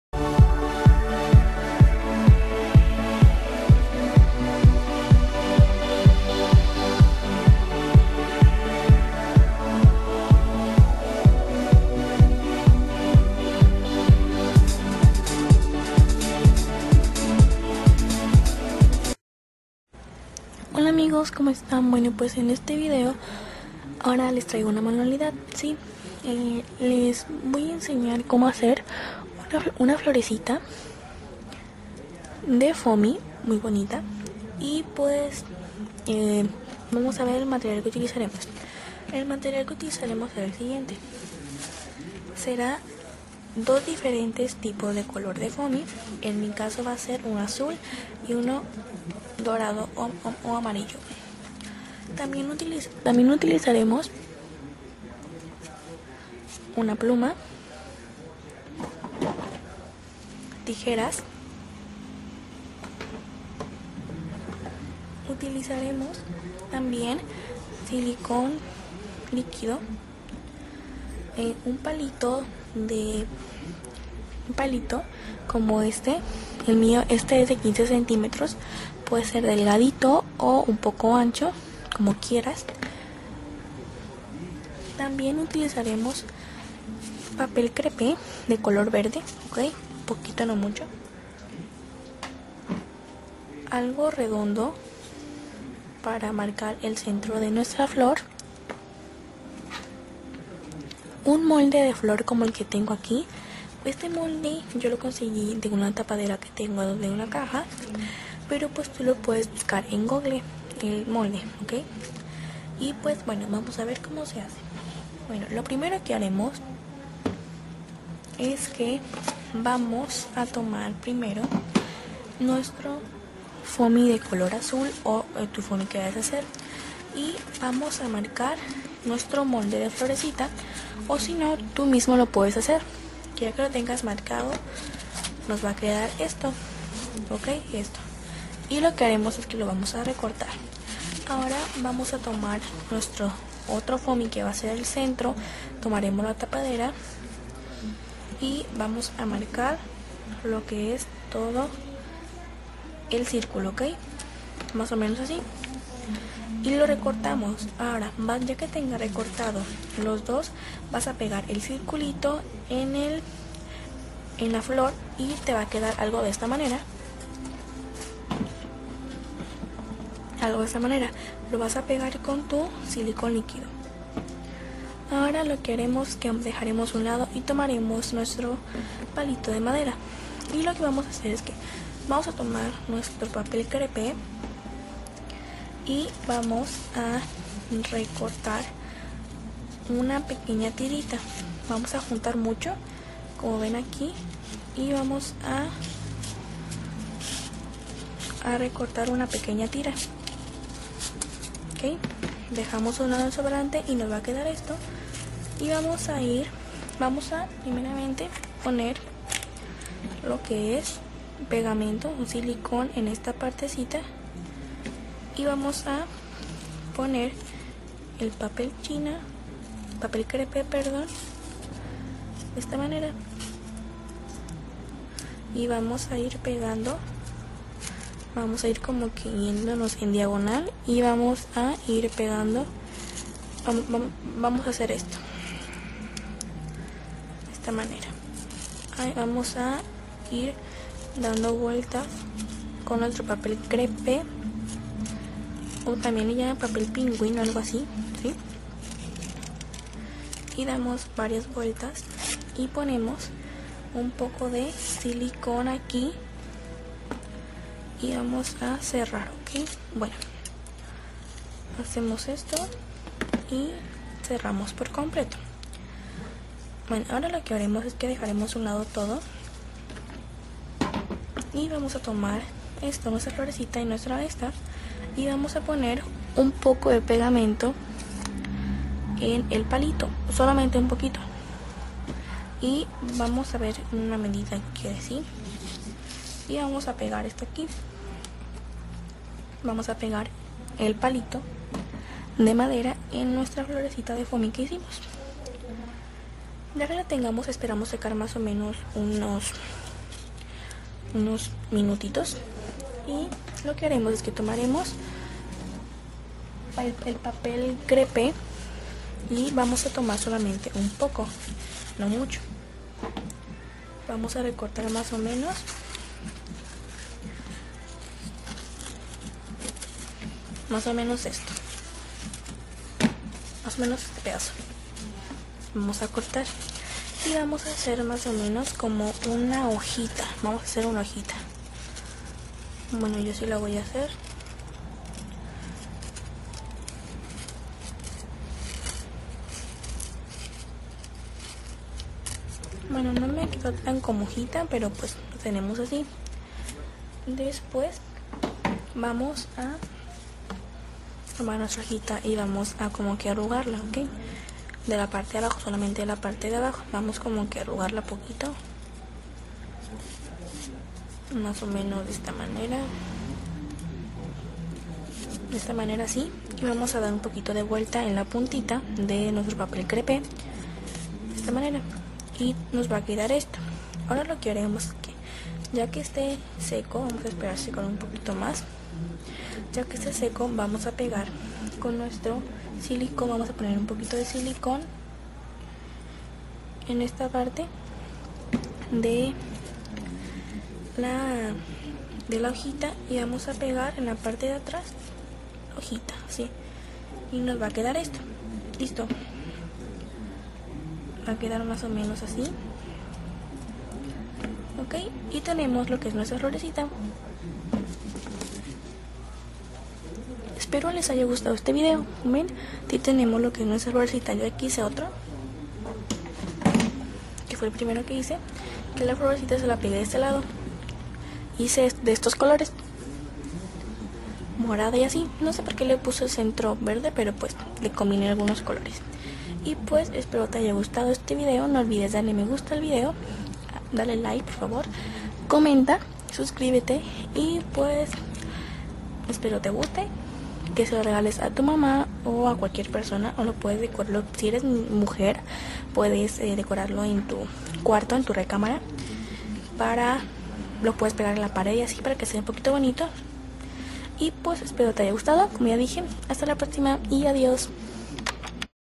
MANUALIDAD:como hacer una florecita de foami para regalar el dia de las madres
Ola!! a qui les traigo este video que es para hacer unas florecitas para el dia de las madres para que se las den a su mamà este 10 de mayo espeor les guste que comenten y que le den like al video les dejo los canales en los que aparesco:

canal colavorativo de manualidadesconalo:

adios!!*